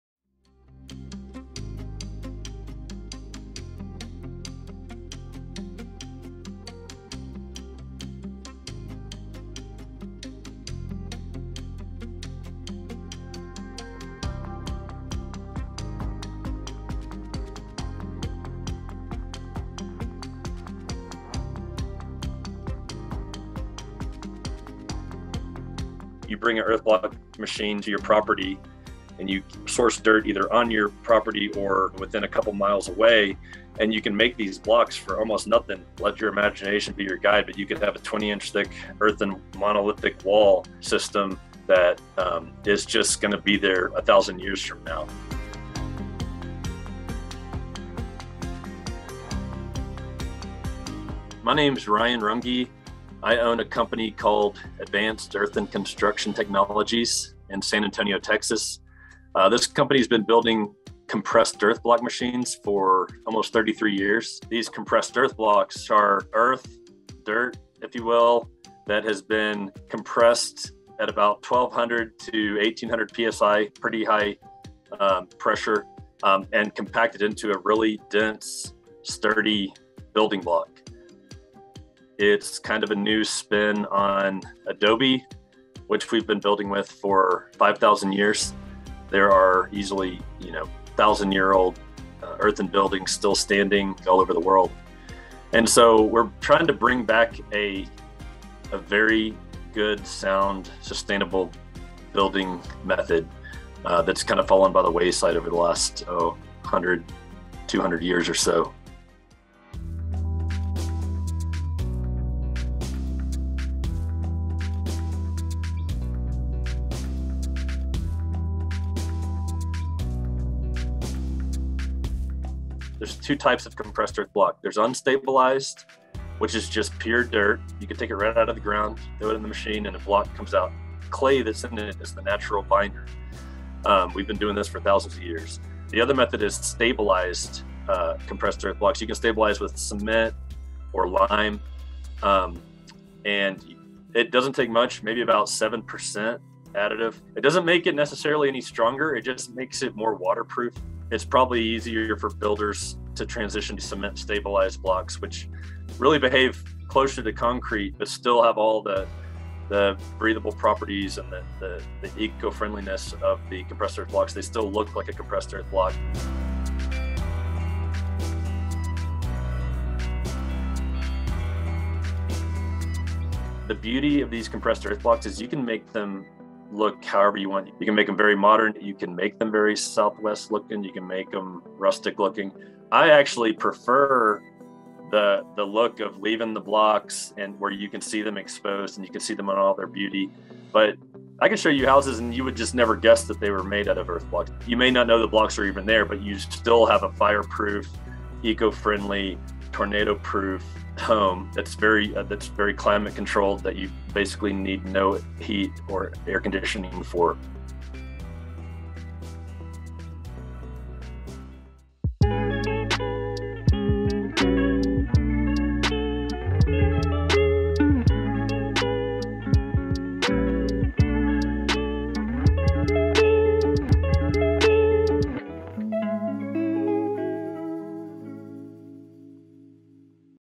10,000-Year-Old Technology Helps These Home Resist Fires and Hurricanes | Mashable
Advanced Earthen Construction Technologies has been building compressed earth block machines for over 30 years. All the machine needs to make the blocks is earth and dirt. Each machine can churn out 480 blocks an hour, giving users enough to build an entire house in one day.